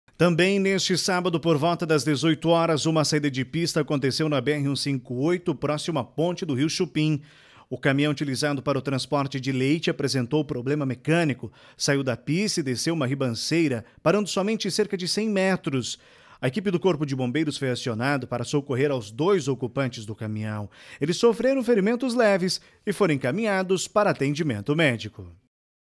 Saída de pista deixa dois feridos na BR-158
Também neste domingo, por volta das 18 horas uma saída de pista aconteceu na BR-158 próximo a ponte do rio Chopim. O caminhão utilizado para o transporte de leite apresentou um problema mecânico, saiu da pista e desceu um barranco parando somente cerca 100 metros.